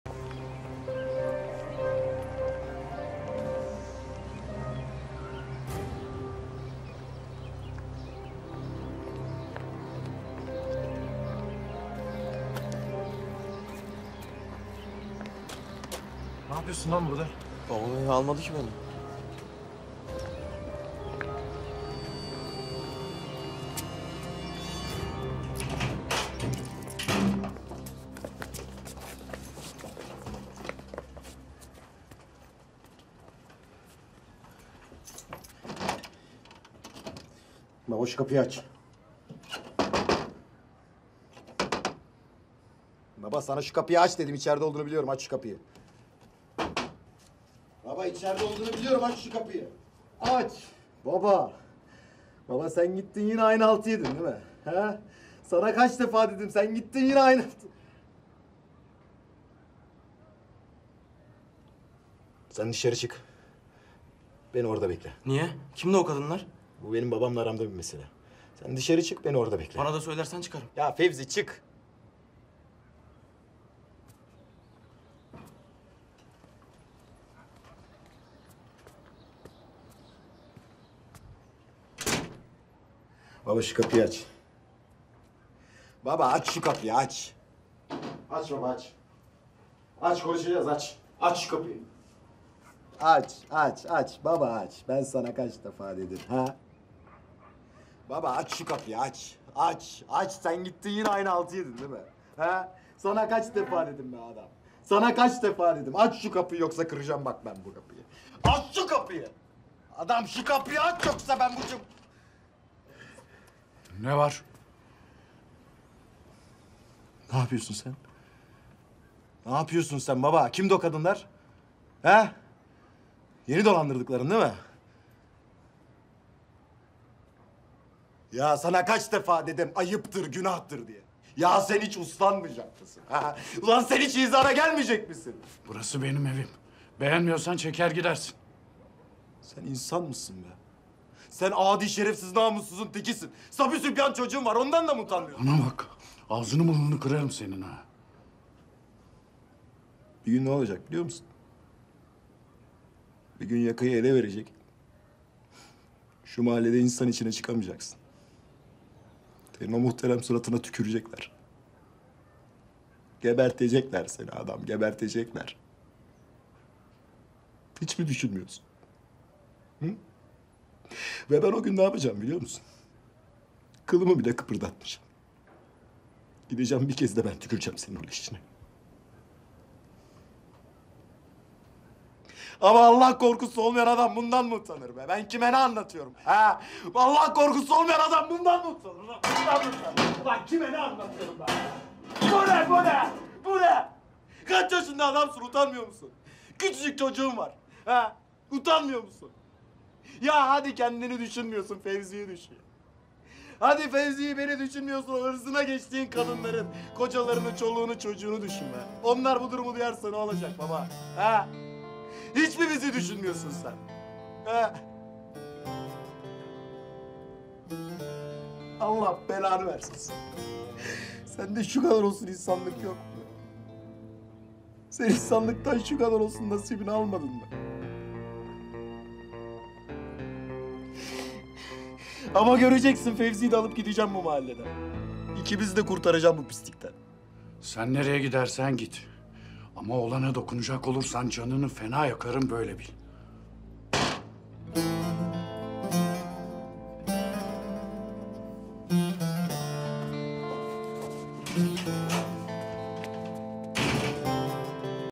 Fevzi'nin yıkıldığı an
Fevzi, babasıyla ilgili gerçeği öğrenince yıkılıyor...